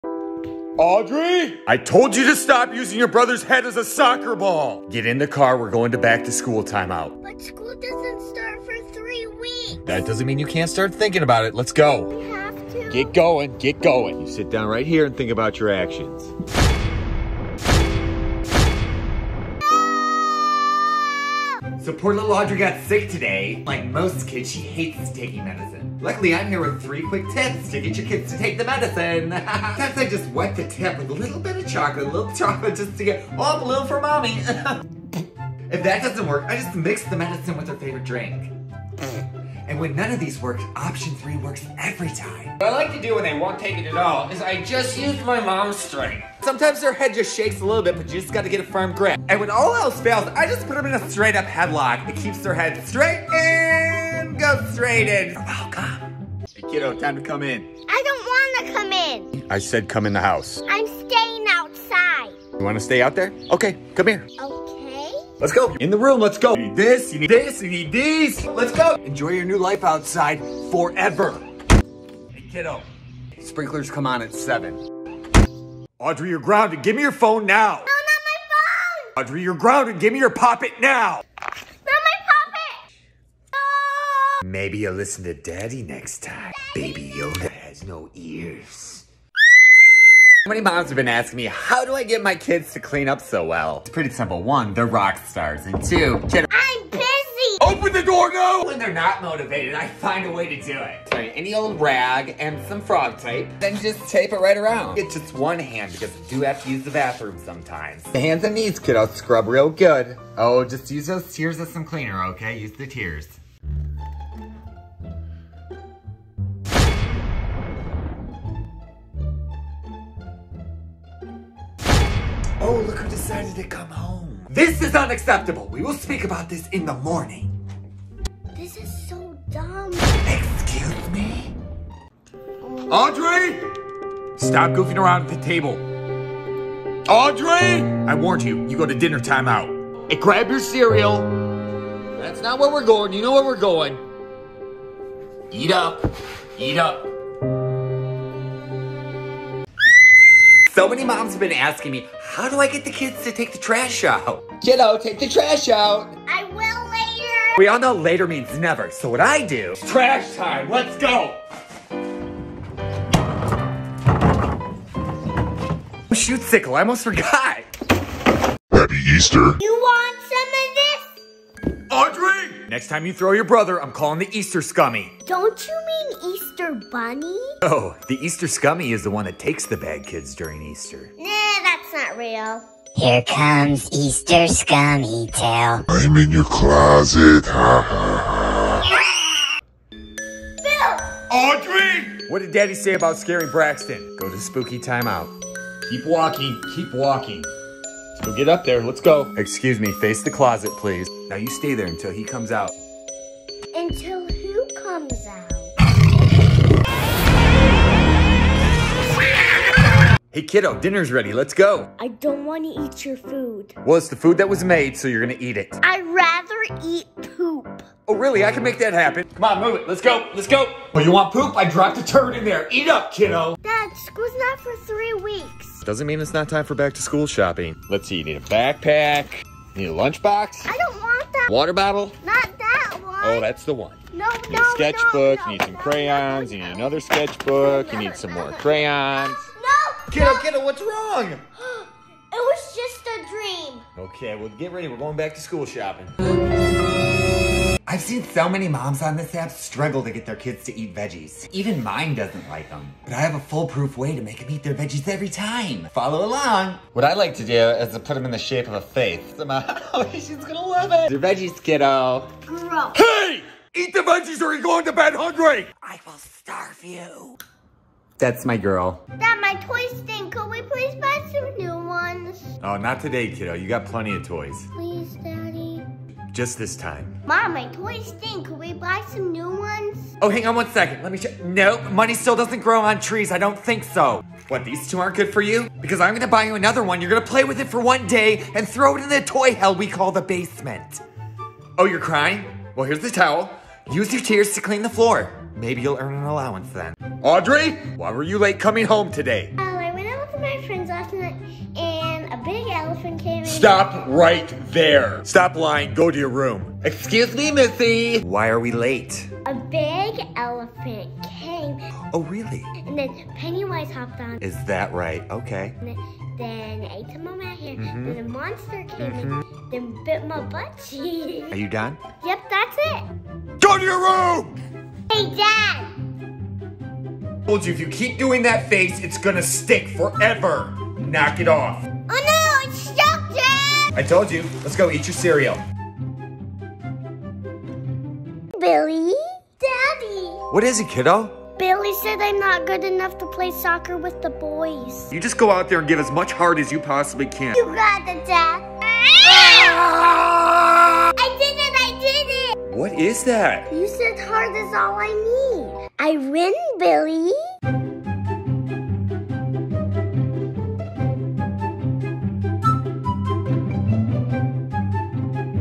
Mom and dad GROUNDED Audri | A funny how to series of parenting tips | themccartys mom vs dad
This hilarious mom vs dad series on how they deal with kids and consequences will have you laughing out loud.  Comedy is king in this house, and we do it right.